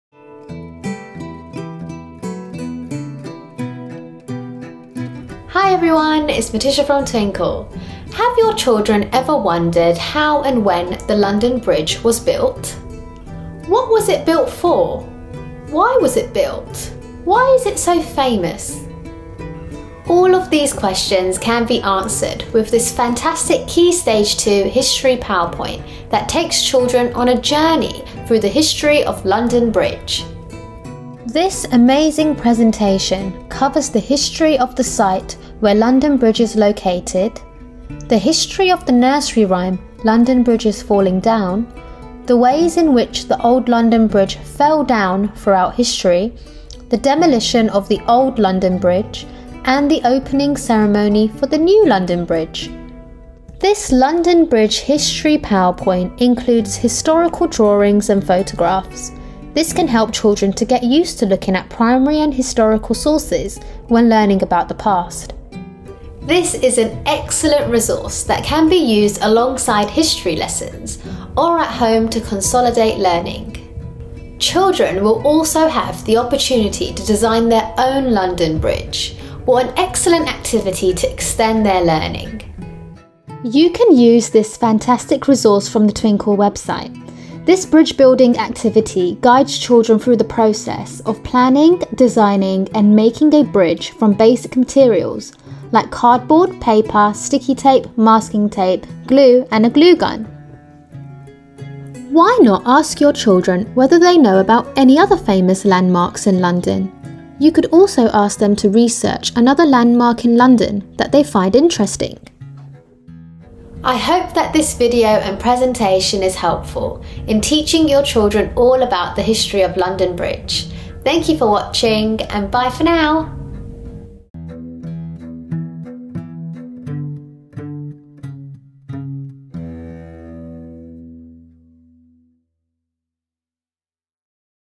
Teaching Your Children All about the History of London Bridge!
In this video, Twinkl Teacher Mitisha, demonstrates how you can teach your children all about London Bridge using our KS2 History of London Bridge PowerPoint:

This fantastic KS2 history PowerPoint takes children on a journey through the history of London Bridge and also includes mini discussion prompts with answers in real time.
You can also use our Structures Planning Designing and Evaluating a Bridge Booklet

This booklet guides children through the process of planning, designing and making a bridge from basic materials.

0:00 Introduction
0:33 All About London Bridge
1:17 How To Design your own London Bridge
1:41 Further learning opportunities